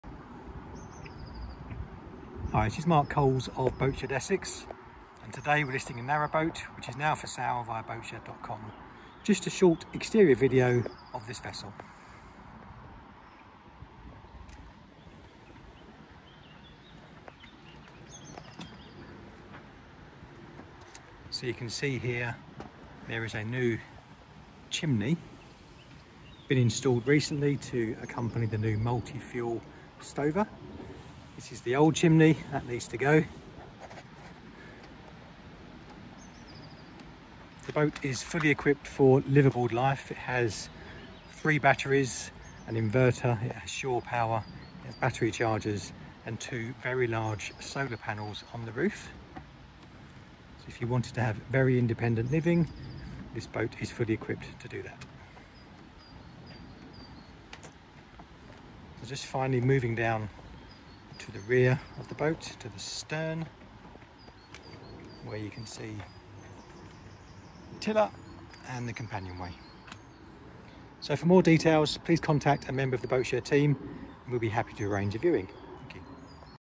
Canal Transport Services Hampton 16m Narrowboat - Boatshed - Boat Ref#332417
Canal Transport Services Hampton 16m Narrowboat for sale with Boatshed Essex Photos and video taken by Boatshed Essex